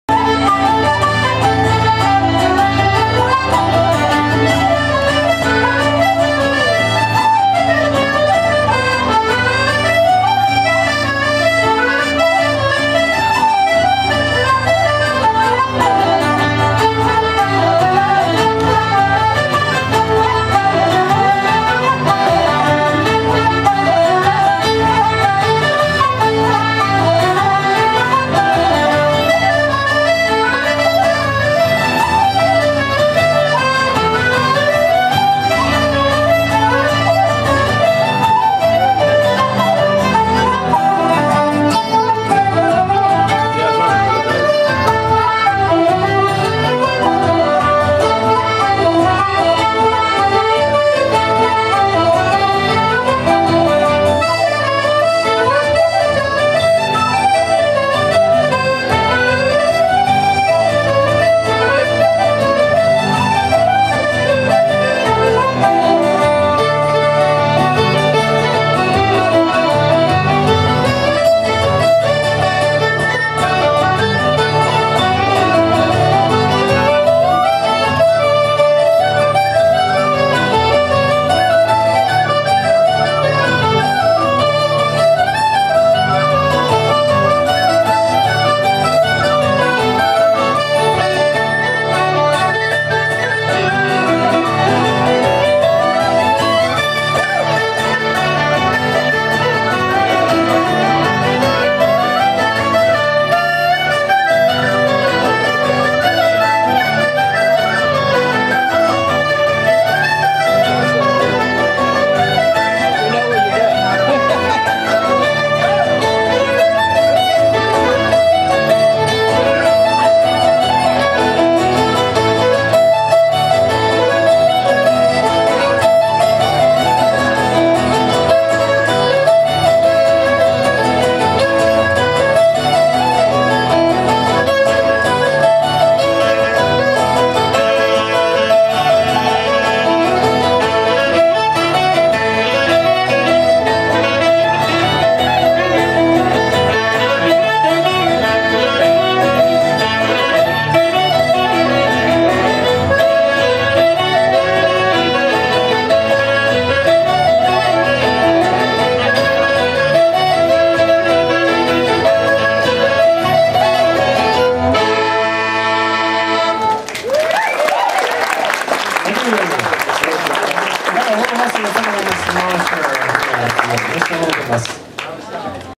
Beyond The Pale - Polkas
Austin Celtic Festival - 11/7/2010.